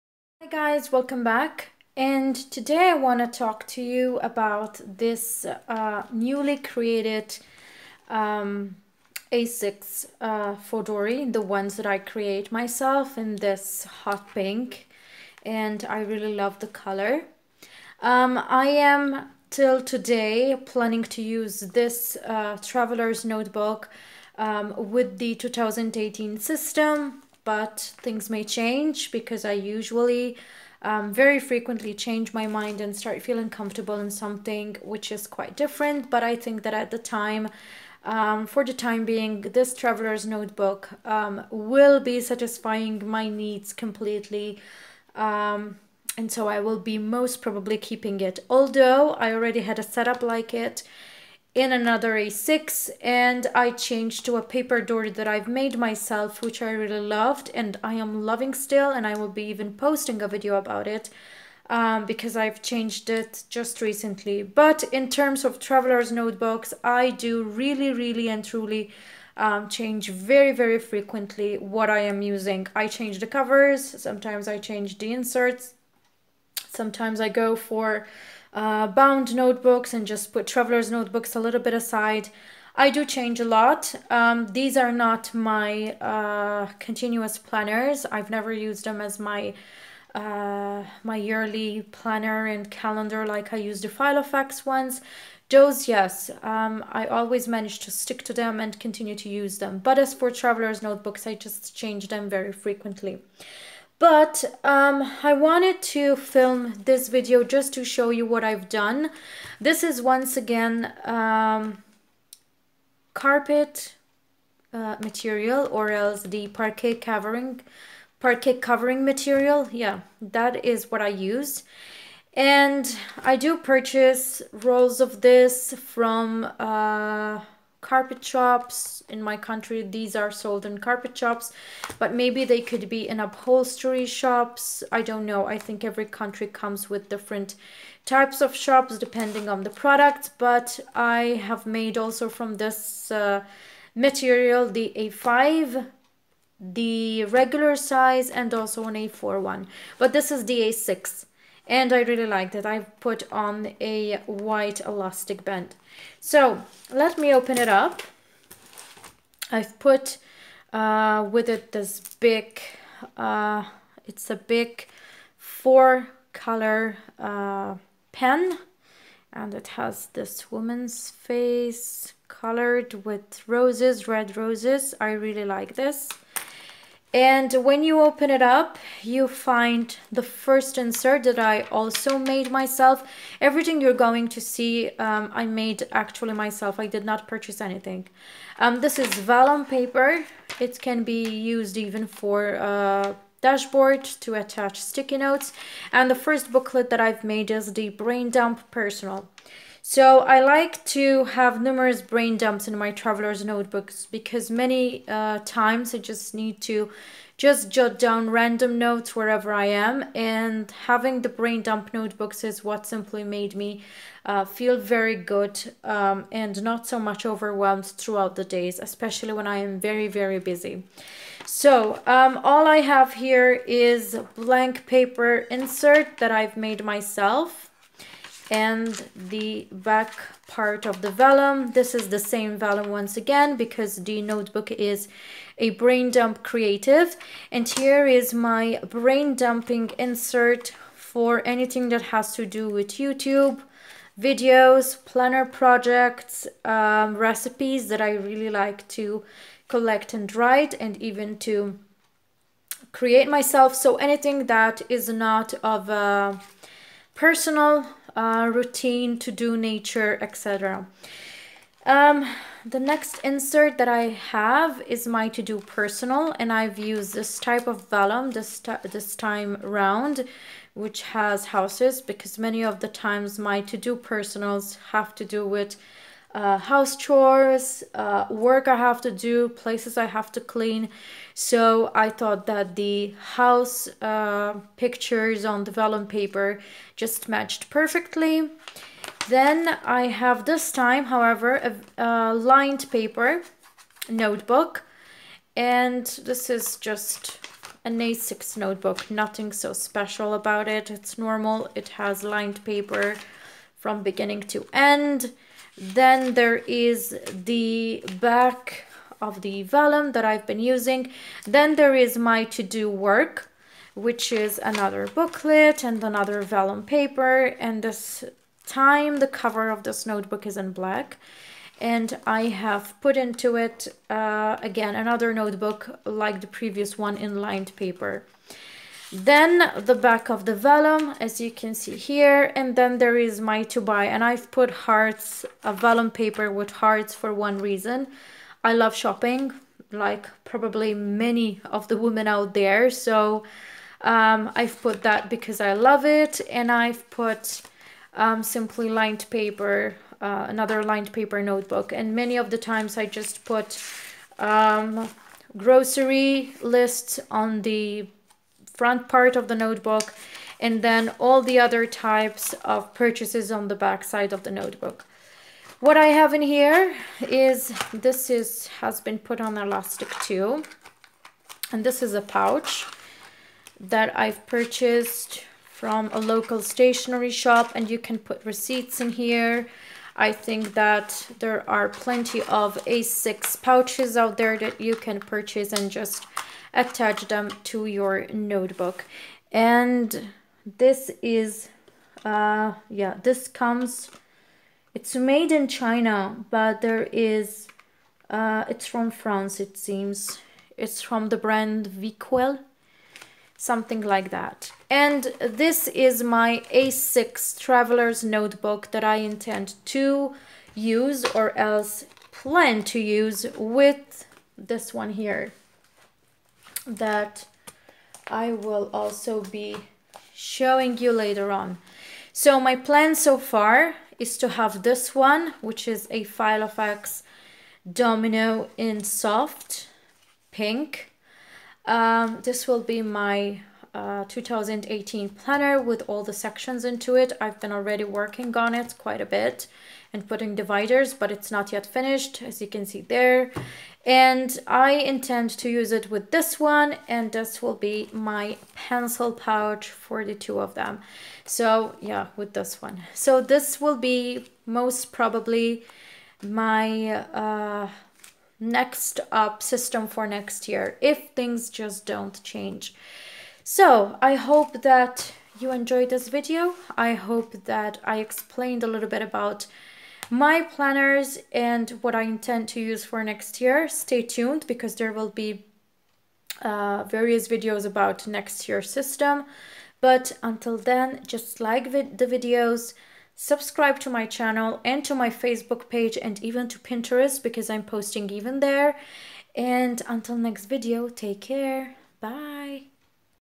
A6 Pink Travelers Notebook
This is part of the Travelers Notebooks episodes that I have been sharing with you. I don't know whether the system will be continued but the A6 Tn is something I am liking. Hope you like this video. XOXO